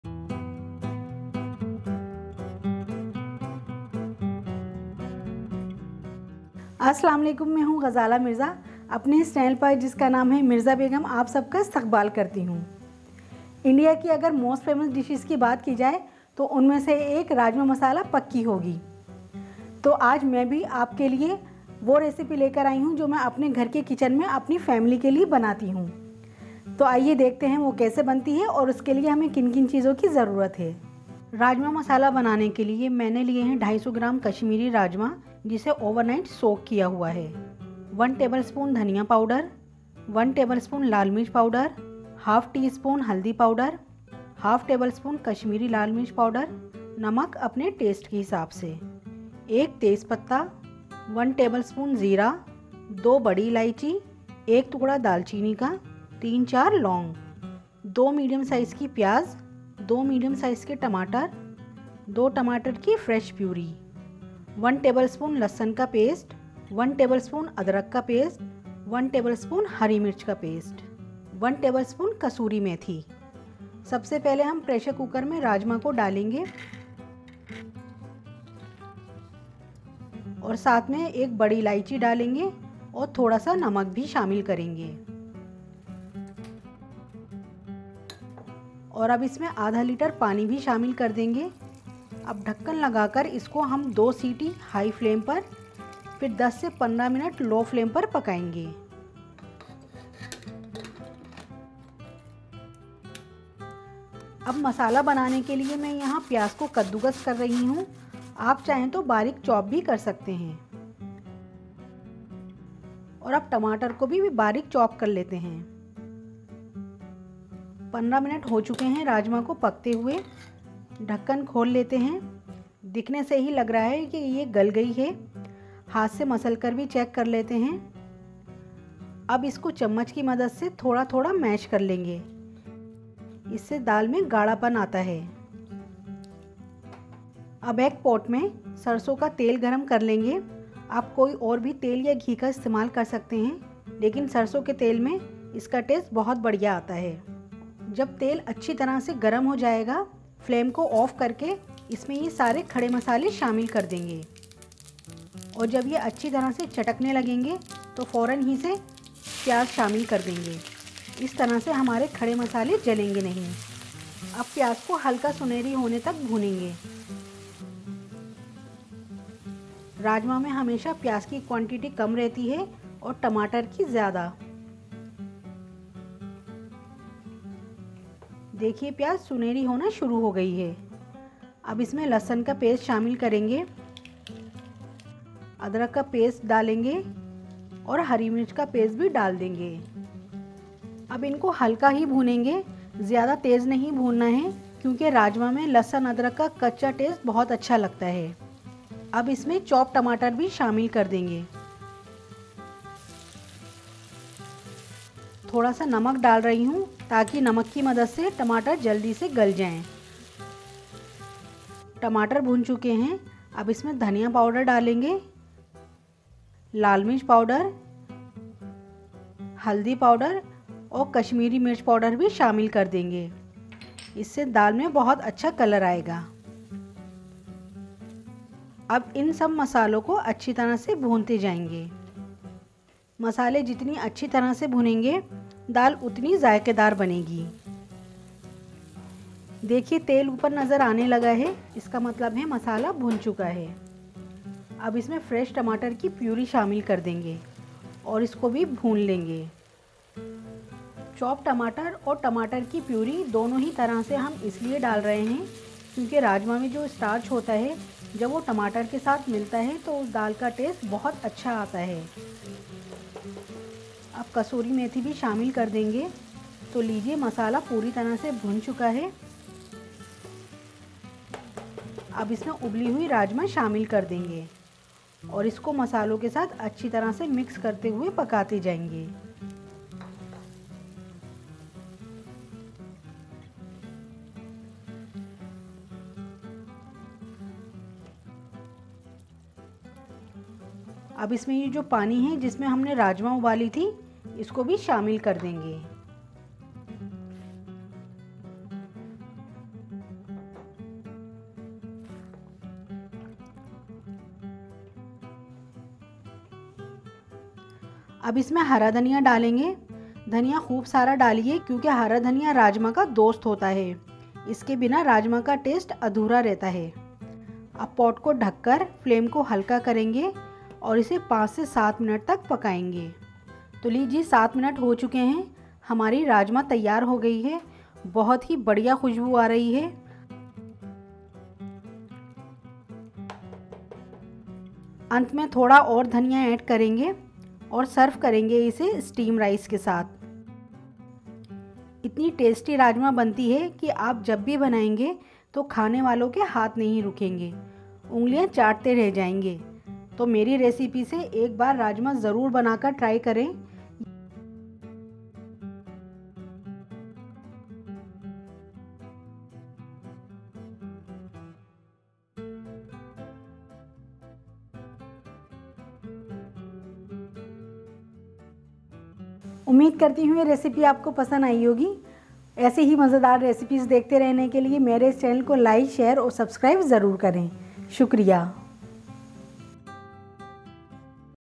Rajma Masala | राजमा मसाला | Quick & Easy Recipe | Delhi's Favourite | Mirza Begum
Whether you're a seasoned cook or a beginner, my recipe of Rajma Masala is sure to impress. So, if you're ready to take your taste buds on a journey to Old Delhi, watch our video and try our mouth-watering recipe for yourself. Your taste buds will thank you!

Music Credit - hoopr.ai

Videography - Subuhi Mirza (Daughter)
Editing - Tayyab Mirza (Husband)